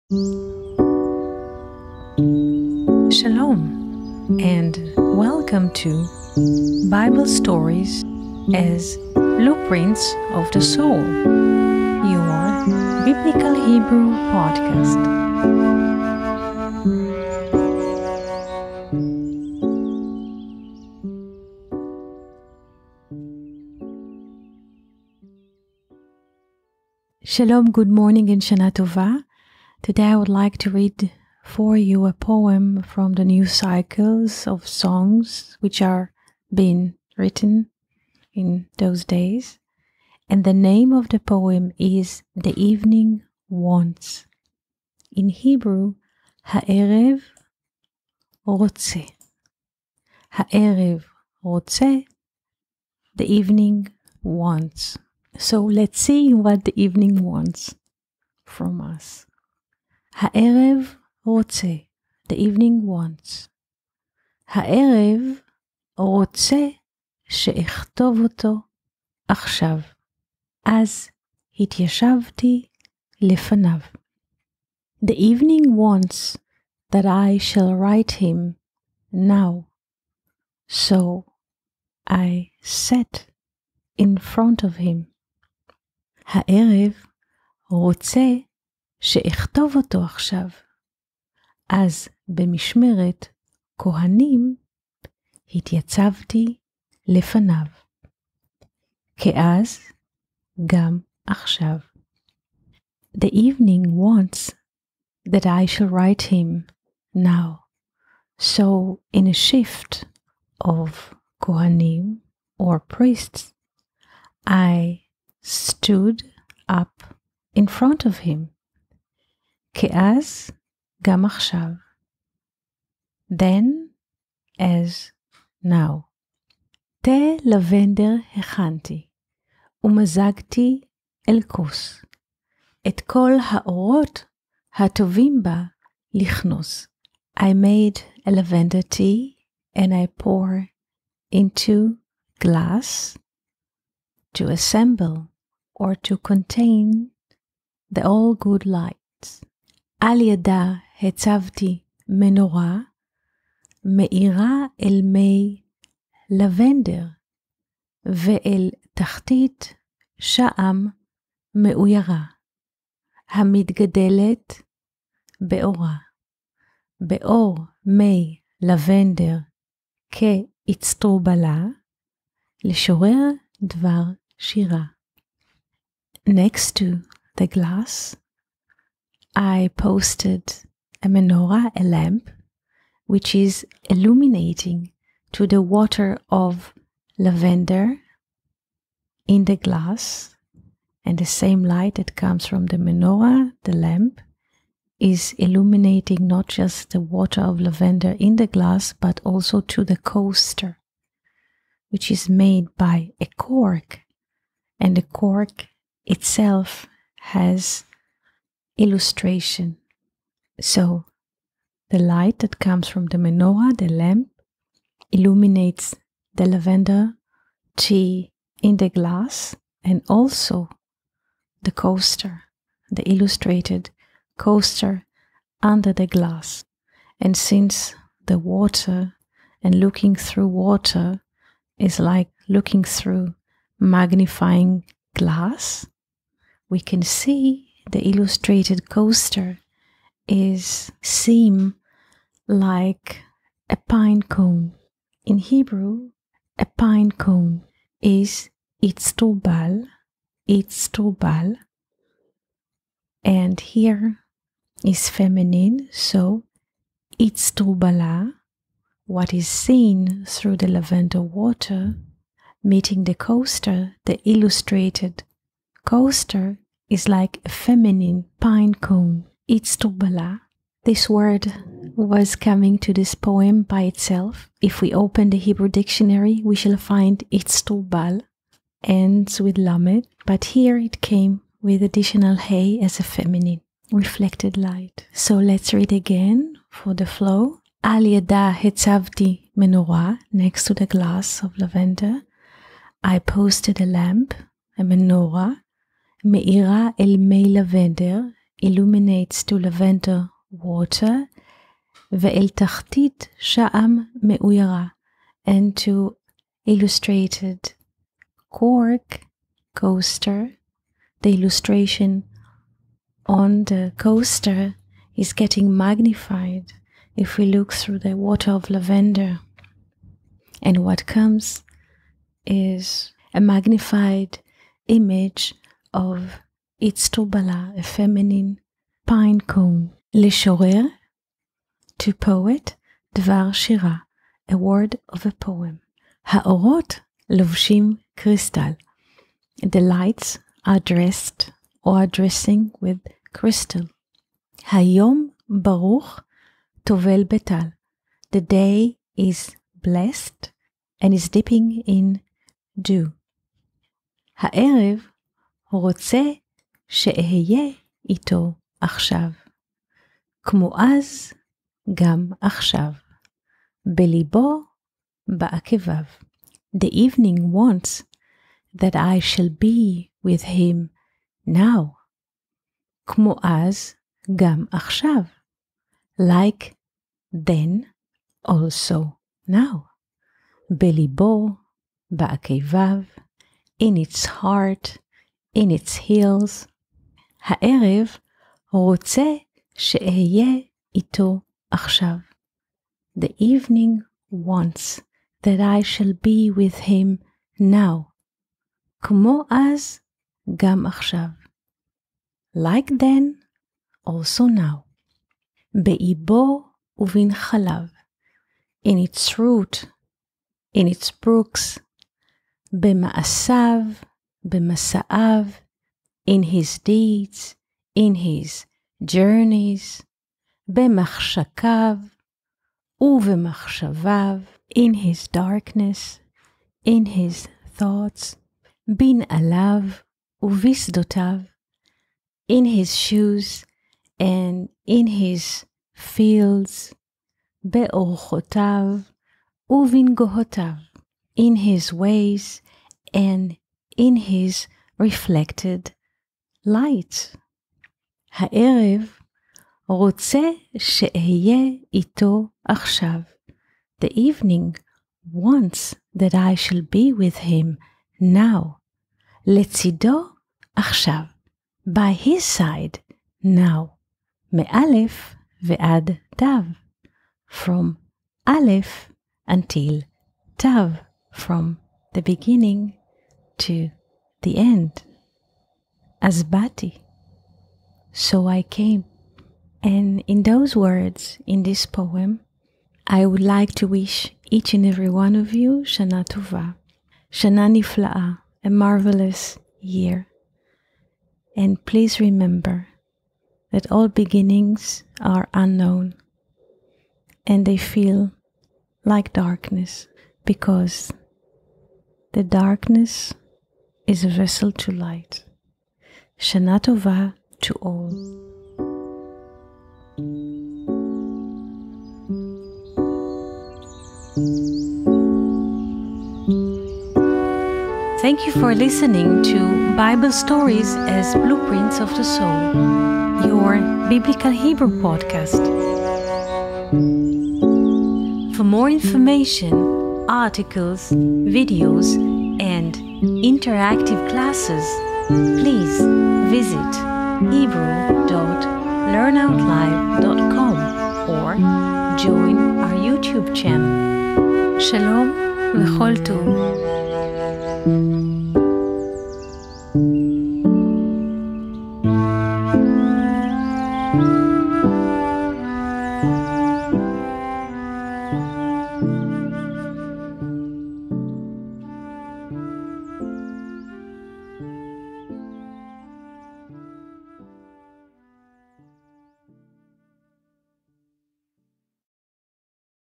The Evening
הָעֶרֶב

הָעֶרֶב רוֹצֶה
שֶׁאֶכְתֹּב אוֹתוֹ עַכְשָׁו
אָז הִתְיַשַּׁבְתִּי לְפָנָיו

הָעֶרֶב רוֹצֶה
שֶׁאֶכְתֹּב אוֹתוֹ עַכְשָׁו
אָז בְּמִשְׁמֶרֶת כֹּהֲנִים
הִתְיַצַּבְתִּי לְפָנָיו
כְּאָז גַּם עַכְשָׁו

תֶּה לָבֶנְדֵּר הֵכַנְתִּי
וּמָזַגְתִּי אֶל כּוֹס
אֶת כָּל הָאוֹרוֹת
הַטּוֹבִים בָּהּ לִכְנוֹס

עַל יָדָהּ הִצַּבְתִּי מְנוֹרָה
מְאִירָה אֶל מֵי לָבֶנְדֵּר
וְאֶל תַּחְתִּית שָׁעָם מְאֻיָּרָהּ
הַמִּתְגַּדֶּלֶת בְּאוֹרָה
בְּאוֹר מֵי לָבֶנְדֵּר - כְּאִצְטְרֻבָּלָהּ
לְשׁוֹרֵר דְּבַר שִׁירָה

הָאוֹרוֹת לוֹבְשִׁים קְרִיסְטָל
הַיּוֹם בָּרוּךְ טוֹבֵל בְּטַל

הָעֶרֶב רוֹצֶה שֶׁאֶהְיֶה אִתּוֹ עַכְשָׁו
כְּמוֹ אָז גַּם עַכְשָׁו
בְּלִבּוֹ, בַּעֲקֵבָיו

בְּאִבּוֹ וּבִנְחָלָיו
בְּמַעֲשָׂיו, בְּמַסָּעָיו
בְּמַחְשַׁכָּיו, וּבְמַחְשָׁבָיו
בִּנְעָלָיו, וּבִשְׂדוֹתָיו
בְּאוֹרְחוֹתָיו, וּבִנְגוֹהוֹתַיו

הָעֶרֶב,
רוֹצֶה שֶׁאֶהְיֶה אִתּוֹ עַכְשָׁו
לְצִדּוֹ עַכְשָׁו
מֵאָלֶף ועַד תָּיו
אָז בָּאתִי

Biblical Hebrew Links & Resources

Koltuv & Shalom